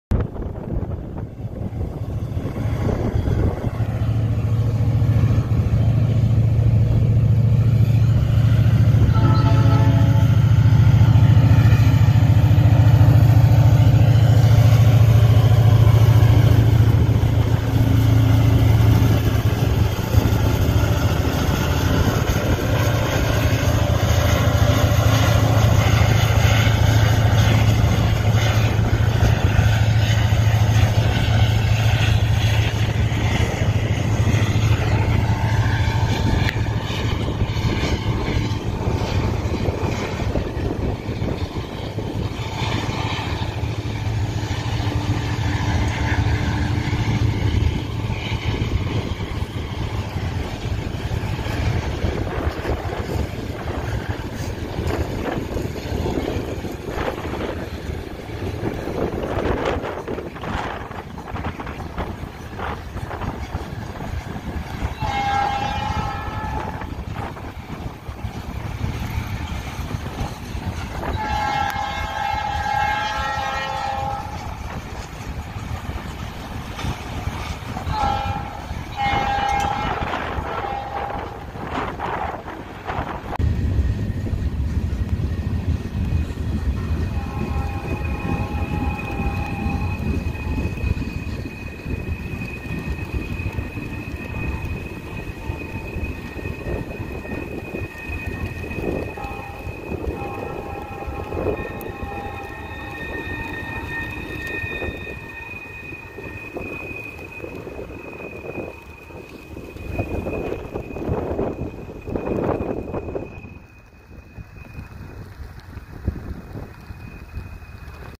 A Low Headlight NS SD40-2 grinds Solo with a coil train!
Filmed on December 30th, 2020.

The Eighth and Final train I saw at Fostoria was NS #3520, an Ex-BN low headlight SD40-2 put solo on the front of this coil train, doing the best it can to pull it at nearly full power. Hope you enjoyed the Fostoria Series!

NS ??

NS #3520: EMD SD40-2: Ex-BN: P5: NS Horsehead.